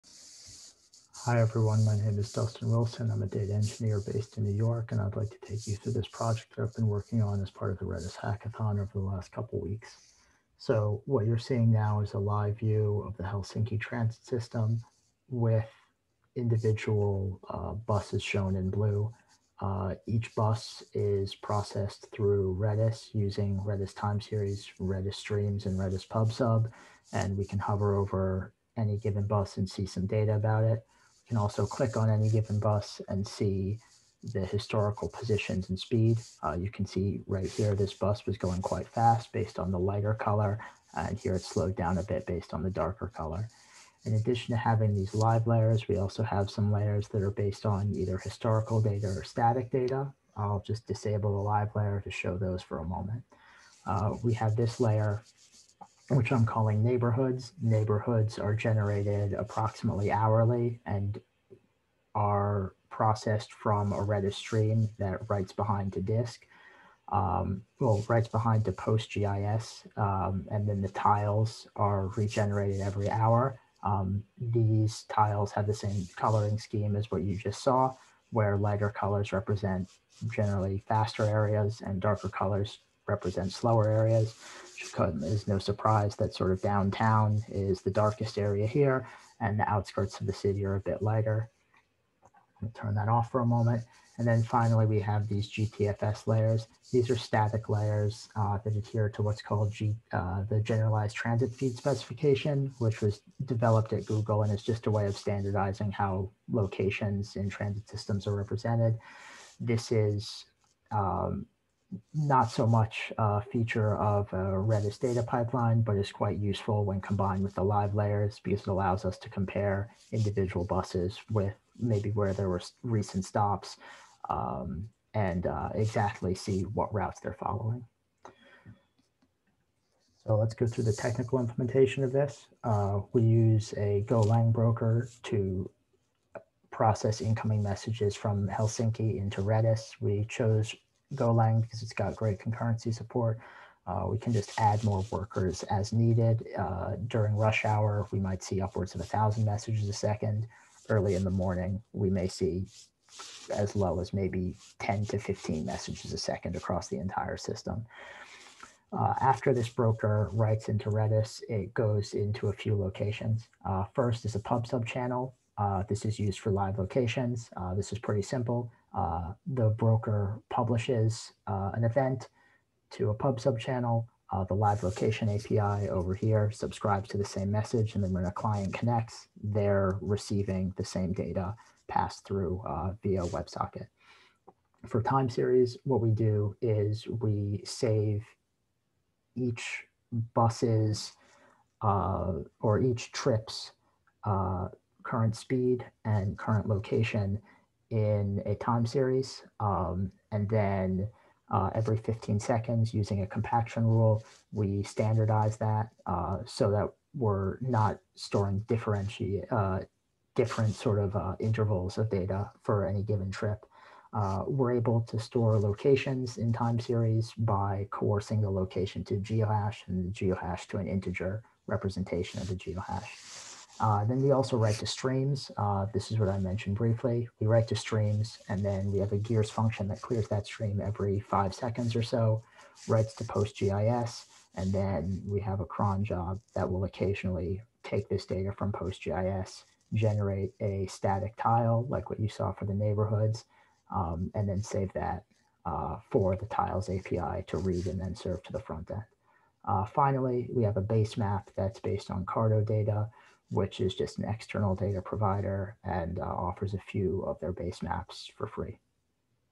Redis Labs Hackathon Submission - Demo Video - Helsinki Metro System Tracking
Submission for Redis Labs Hackathon. This application uses Redis (Gears, PubSub, TimeSeries), PostGIS, and Tippecanoe to present a live view of the Helsinki Metro System.